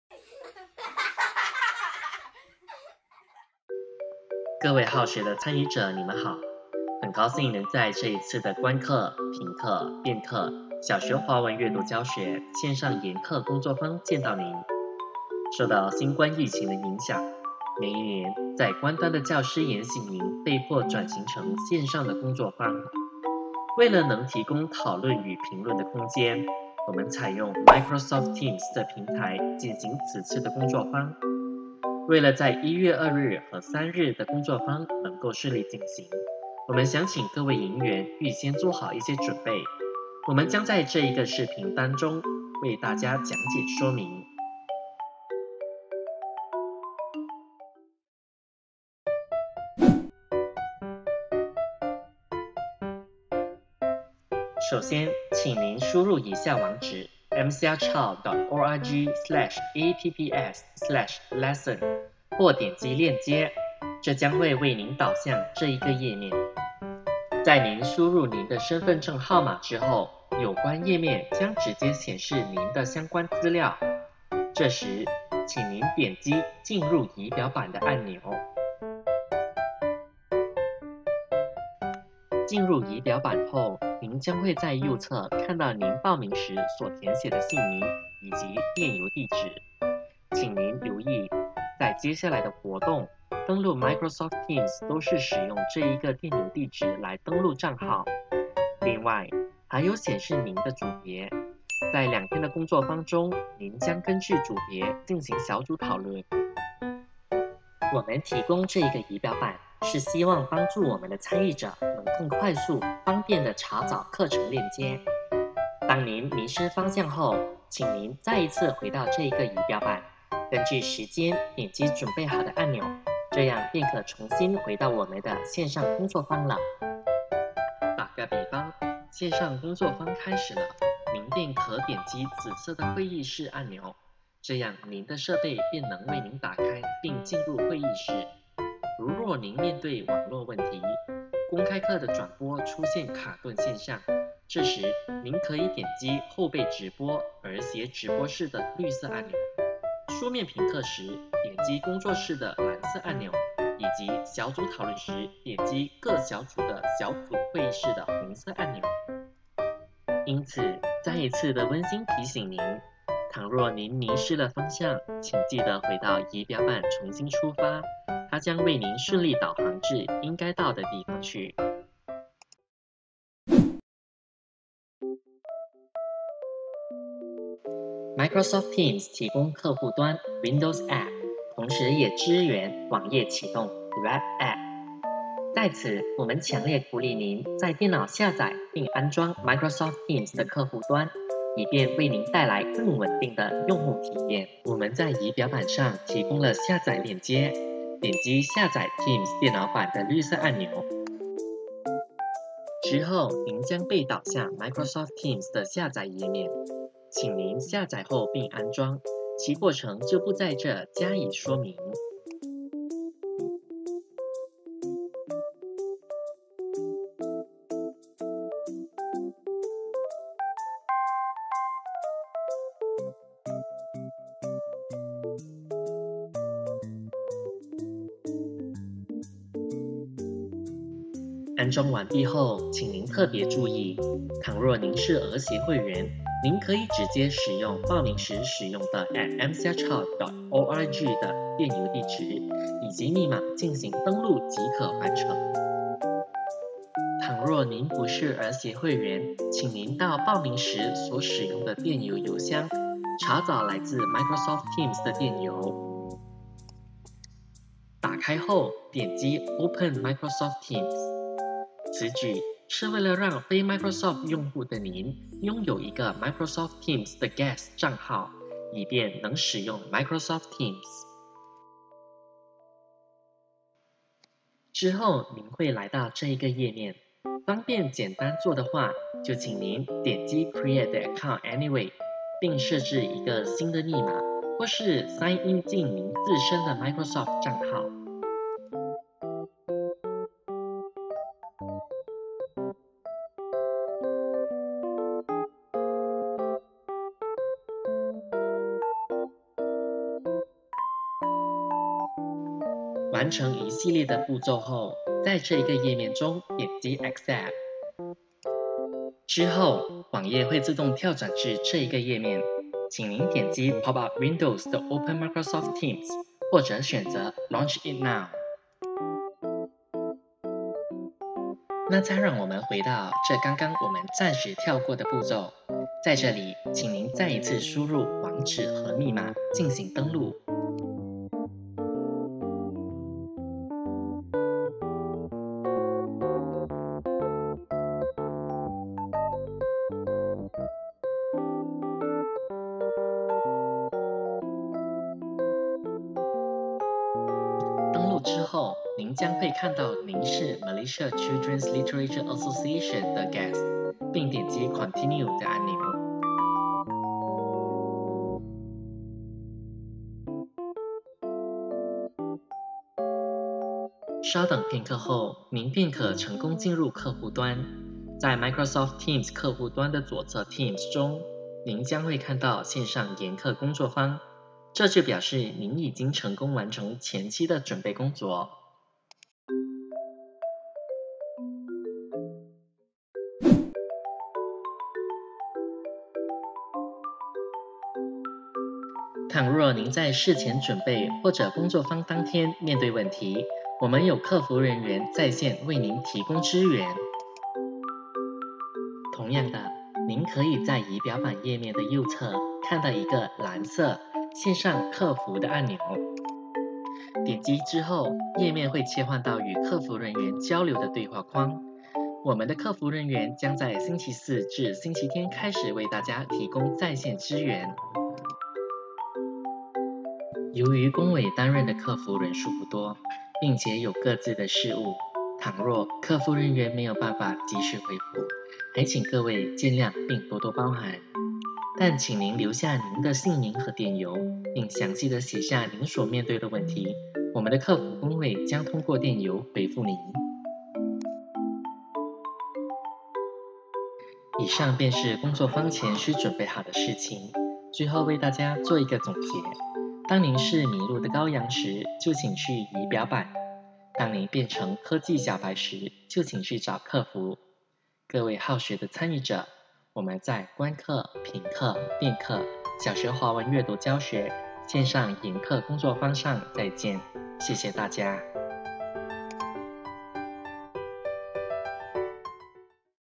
【线上研课工作坊】营前须知 ｜马来西亚儿童文学协会
【观课·评课·辩课】
小学华文阅读教学——线上研课工作坊
受到新冠疫情的影响，每一年在关丹的教师研习营被迫转型成线上的工作坊。为了能提供讨论与评论空间，我们采用Microsoft Team的平台进行此次的工作坊。因为，为了在1月2日和3日的工作坊能够顺利进行，我们想请各位营员预先做好一些准备。我们将在这一个视频当中为大家讲解说明。
0:00 Intro
0:49 仪表板
2:53 下载及安装Microsoft Teams
6:23 客服支援

音频：陈昱伸
录制：游智勇
剪接：姚净元